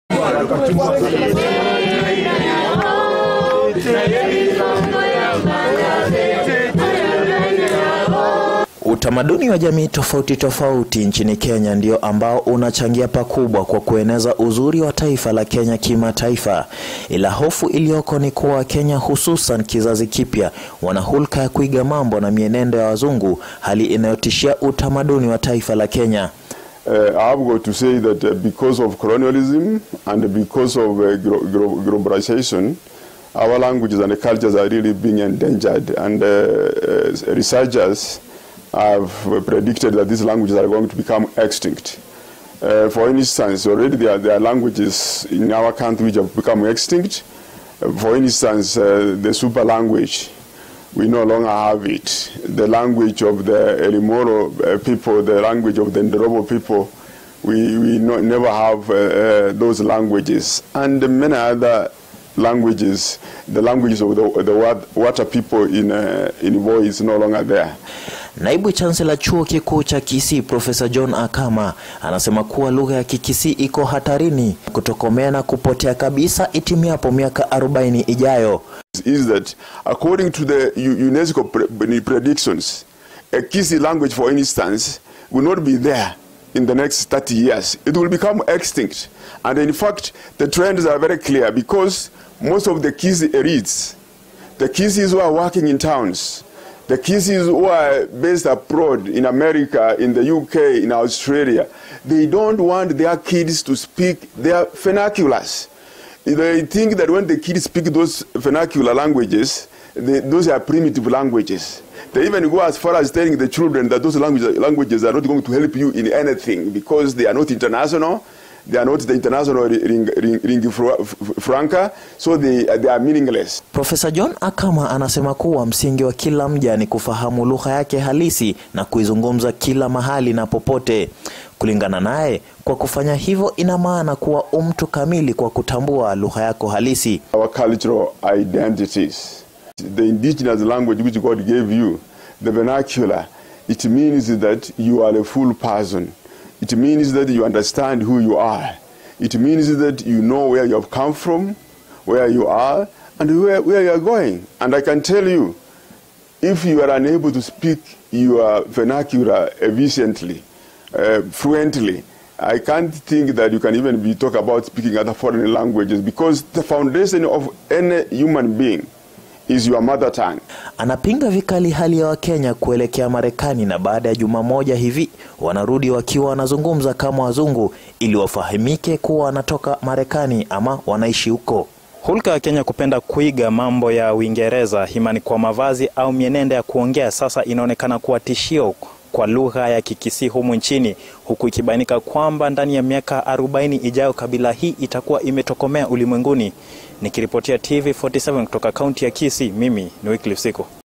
No more kisii language in the next 40 years.
In the next 40 years, the Kisii language will be extinct, Why do you think that the kisii language will be no more